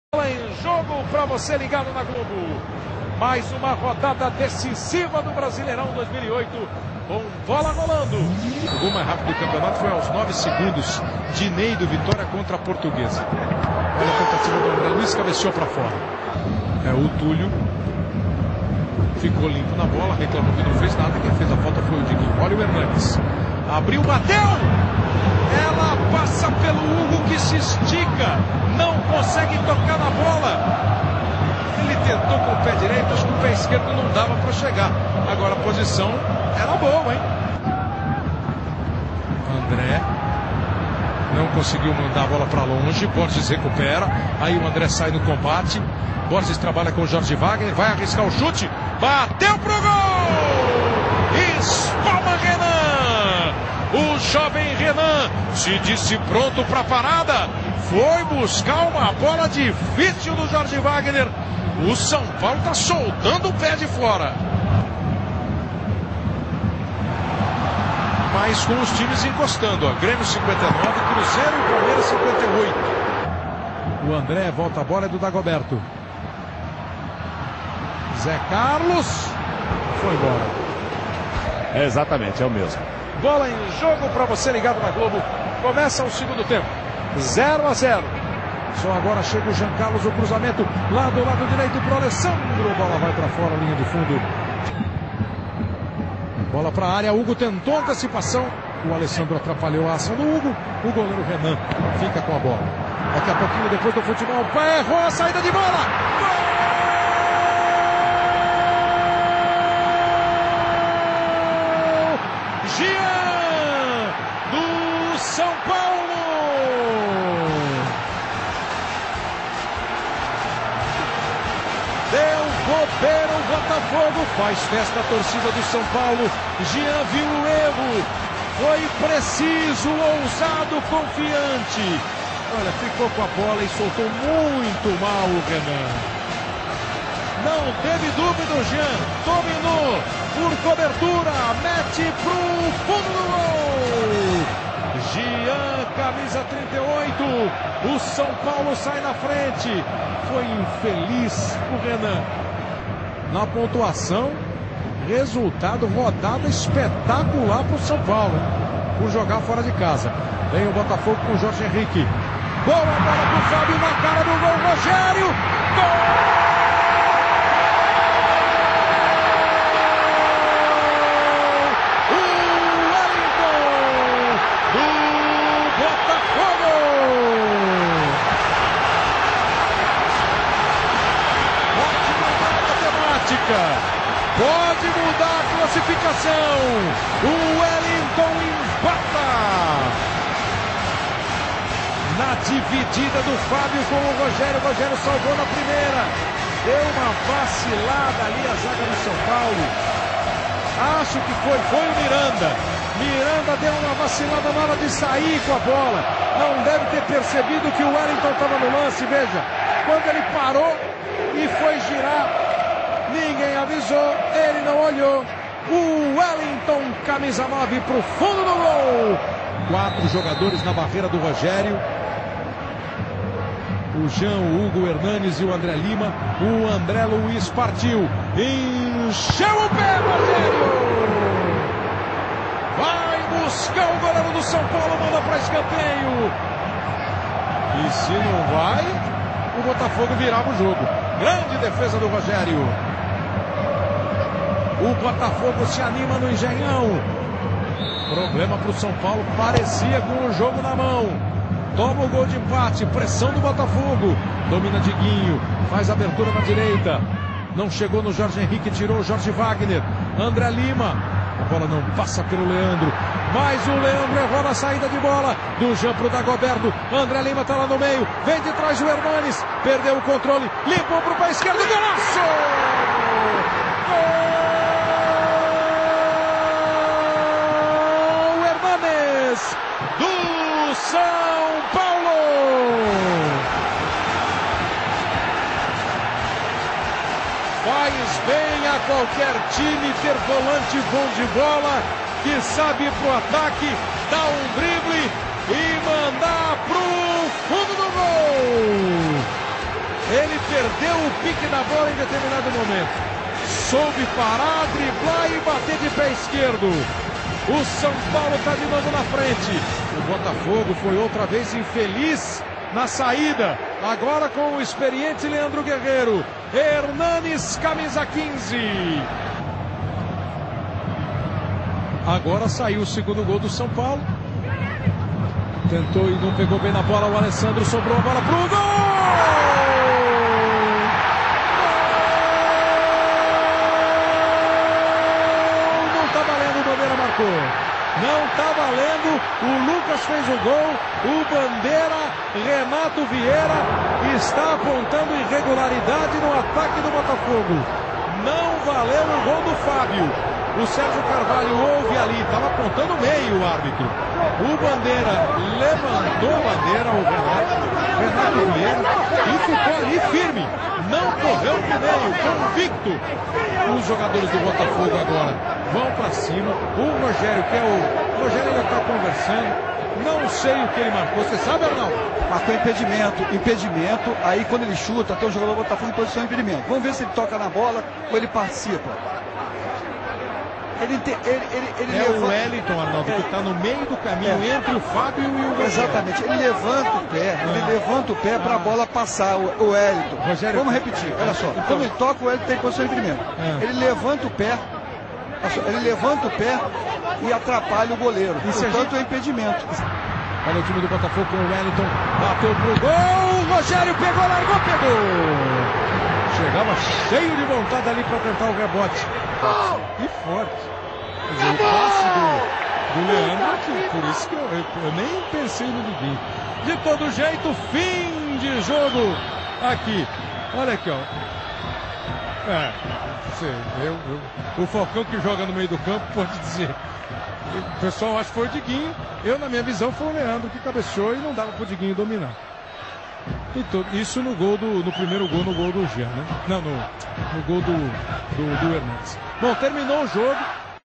AJ TV Melhores Momentos 2008 - Botafogo 1 x 2 São Paulo
Na 32ª rodada o SPFC se supera de novo e encosta de vez na ponta.